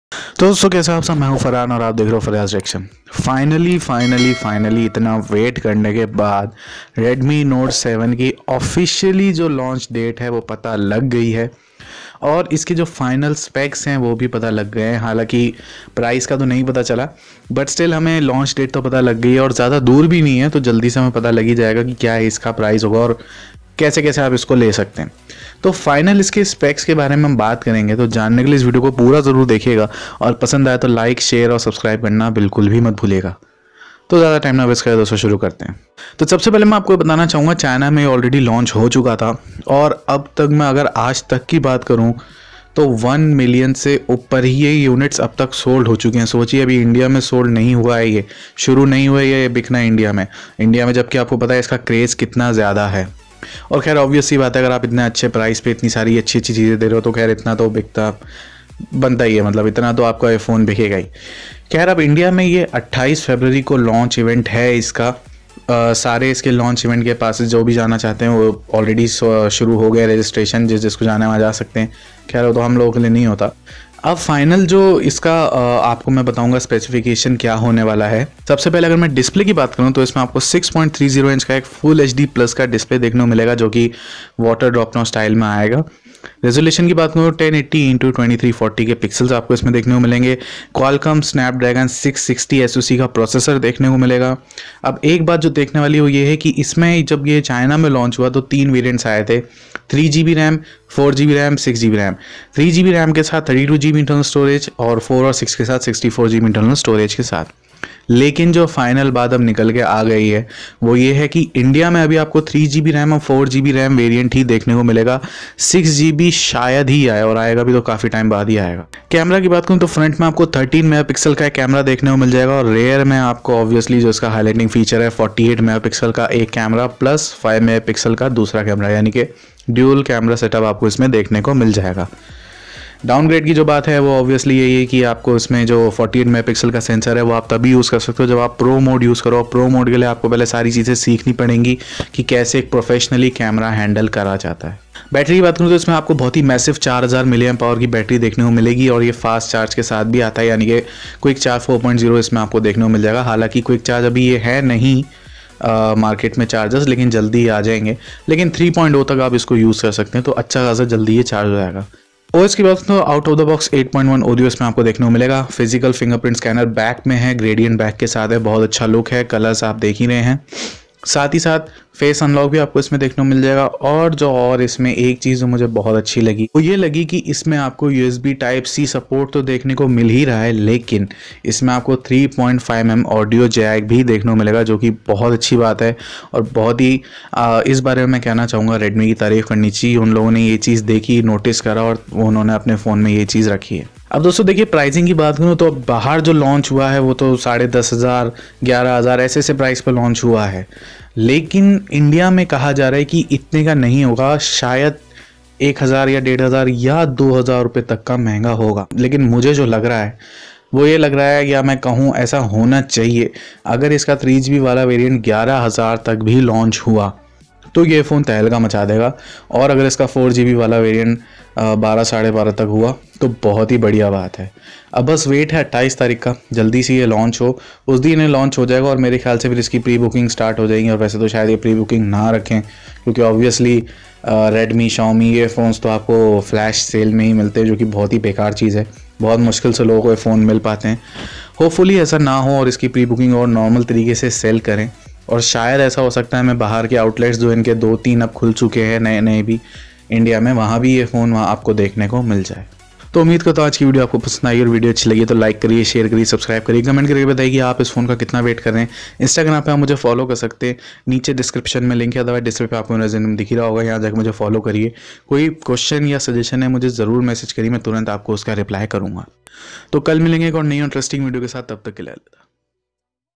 REDMI NOTE 7 OFFICIAL LAUNCH DATE AND SPECS REVEALED || ALL DETAILS
TO DOSTO AAJ HUM BAAT KRENGE REDMI NOTE 7 KI FINAL RELEASE DATE OR FINAL SPECS KE BARE ME..FINALLY PATA LAG GAYE HAI TO JANNE KE LIYE ISS VIDEO KO PURA DEKHE..

Thanks
FAREHA'S REACTION
Farehas Reaction